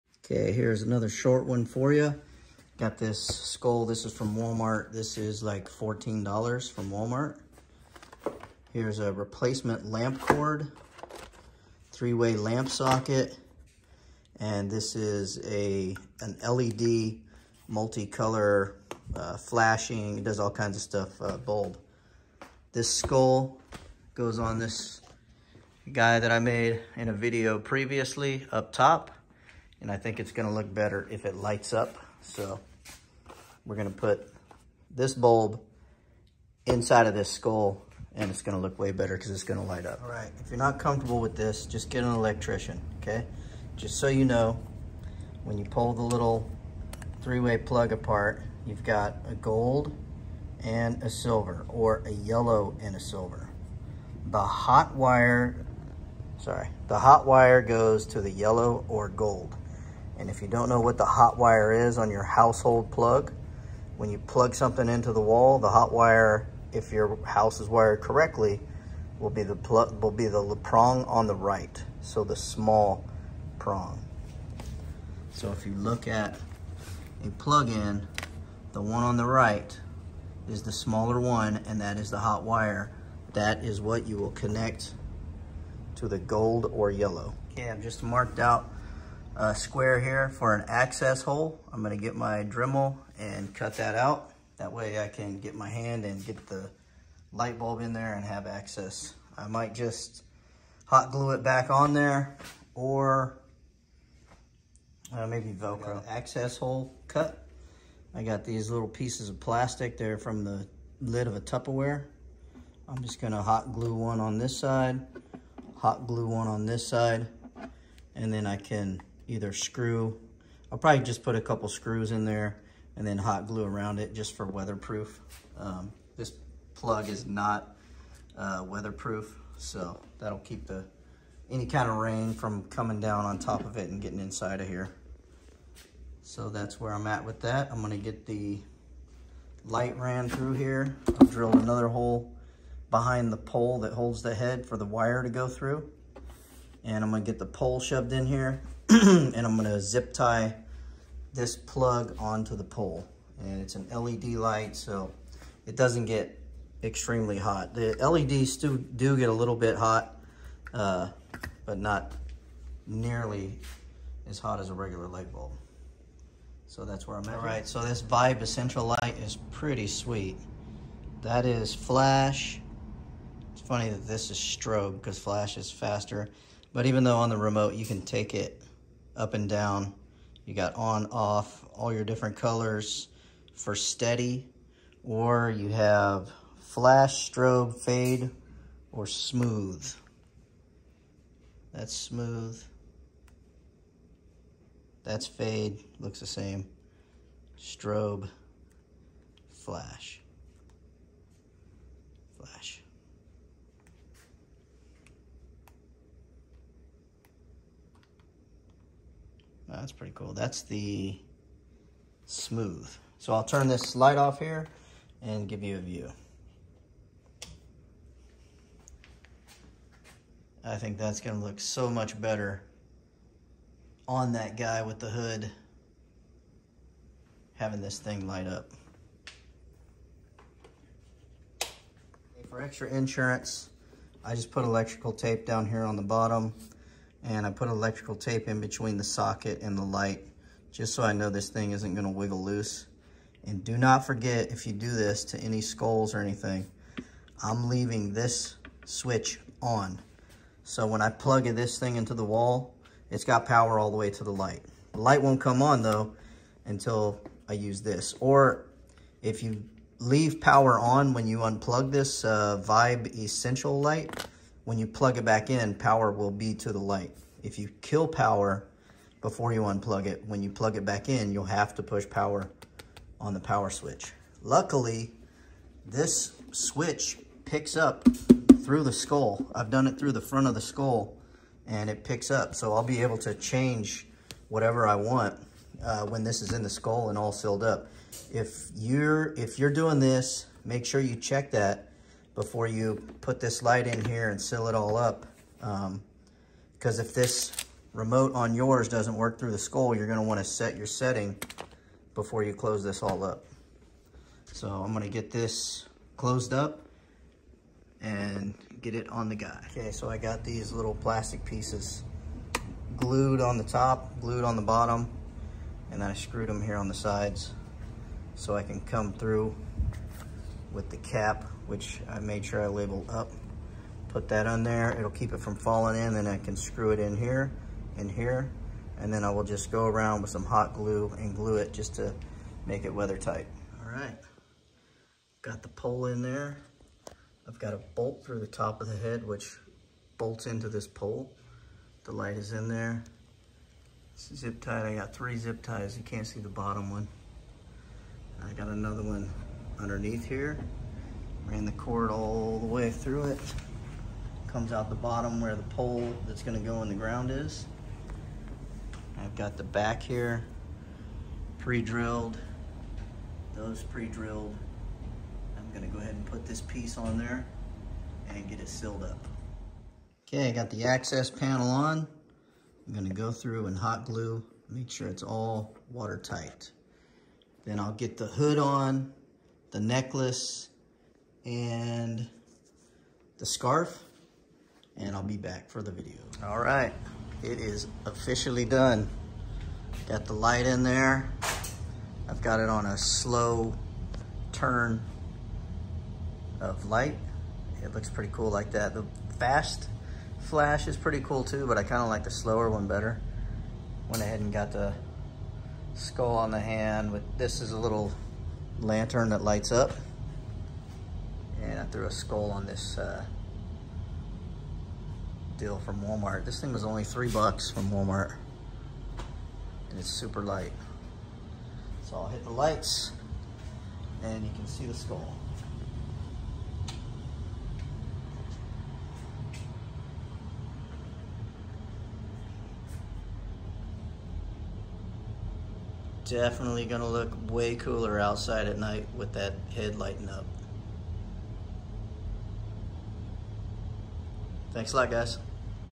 🎃 Adding a light to a skull for extra Halloween Awesomeness 👻
A short tutorial of adding a color changing light to a skull prop that will make your yard display that much better.  I got the parts and the skull from Walmart and I will link them in the description.  Hopefully this helps or inspires someone because it really looks great at night.  Unfortunately I cant find the skull on Walmart's website.

lamp socket=